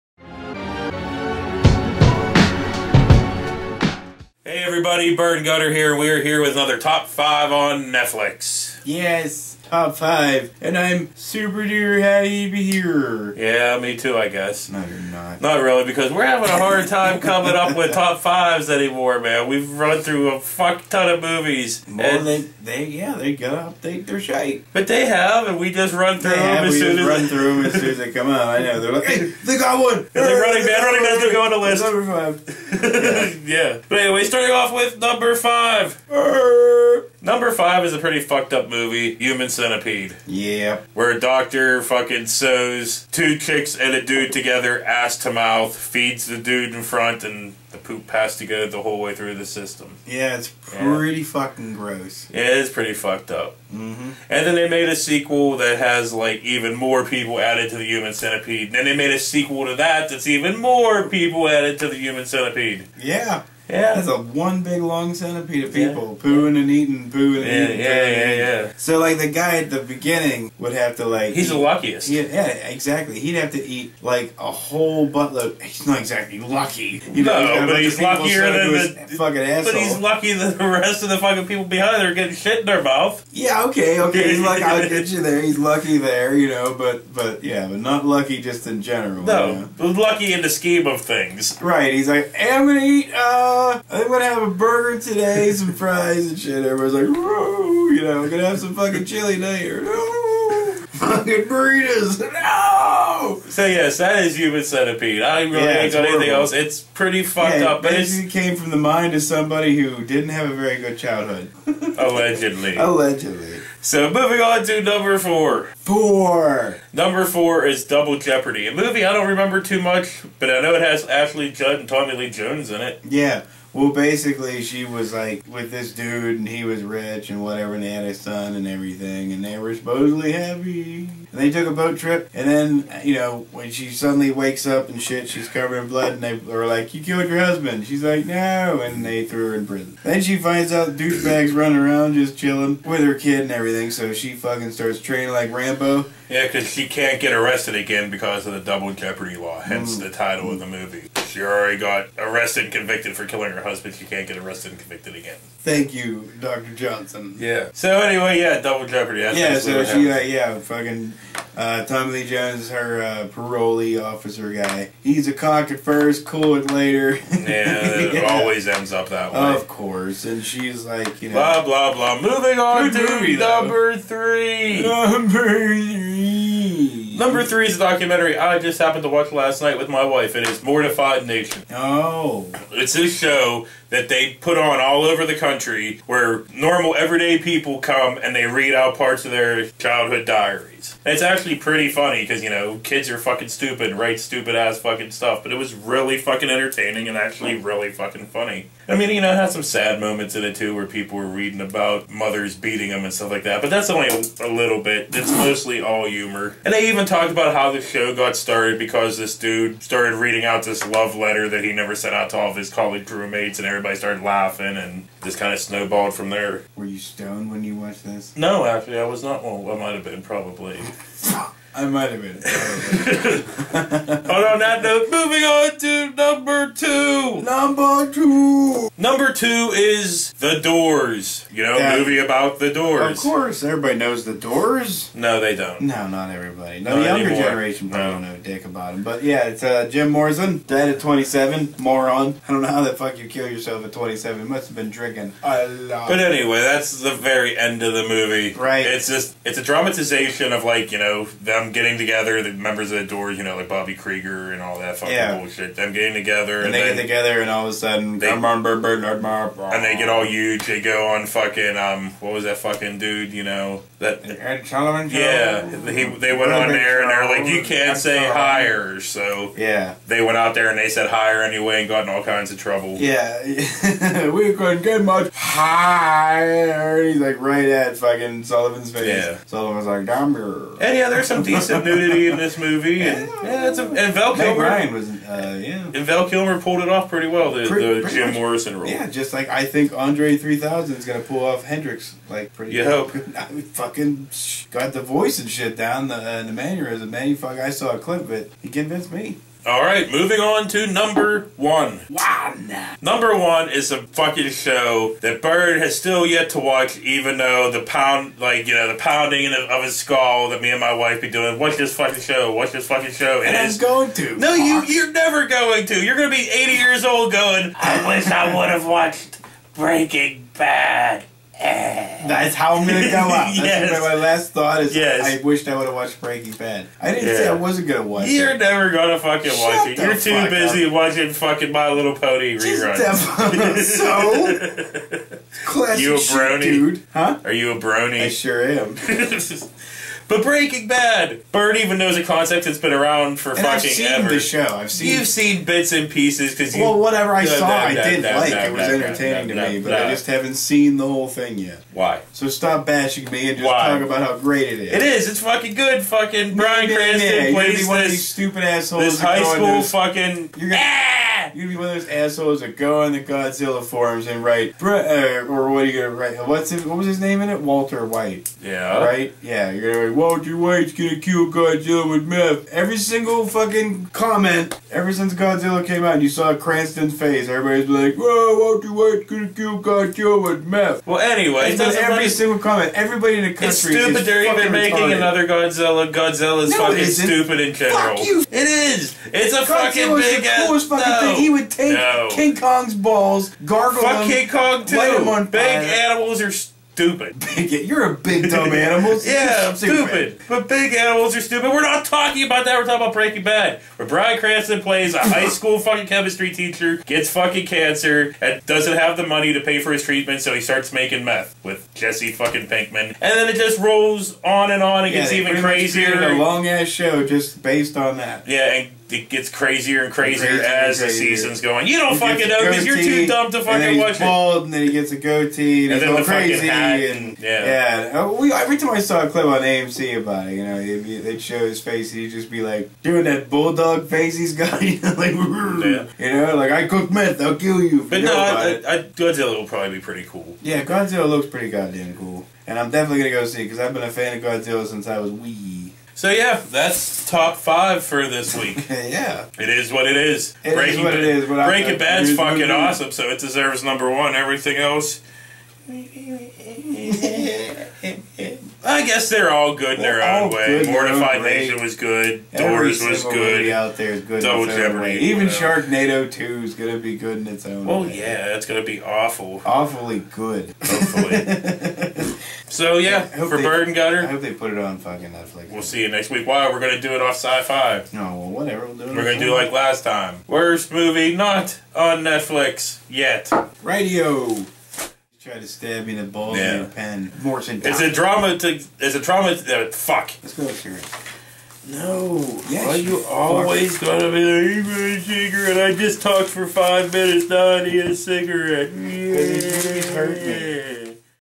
Top 5 On Netflix episode 52
Bird & Gutter count down another Top 5 streaming on Netflix.